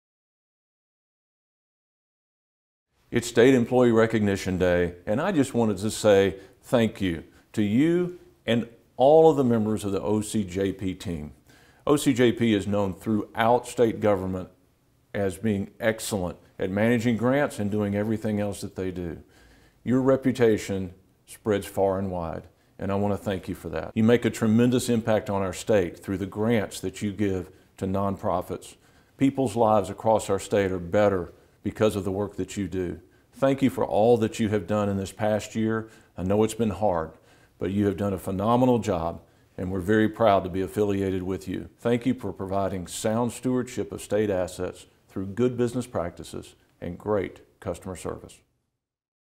F&A Office of Criminal Justice Programs Appreciation
F&A Commissioner Jim Bryson's message to the Office of Criminal Justice Programs on State Employee Recognition Day 2023.